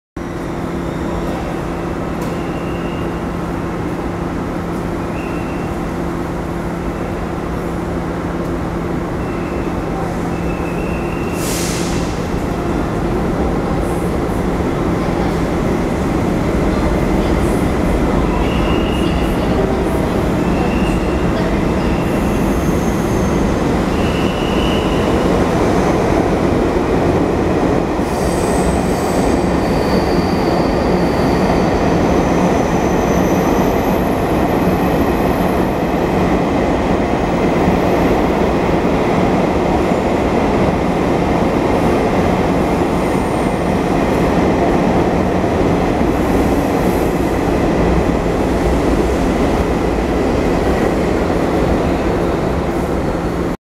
[掛補]台鐵PP自強號+E308通過(桃園)
[掛補]台鐵PP自強號+E308通過(桃園),攝於2015年7月28日,來拍攝桃園臨時新站~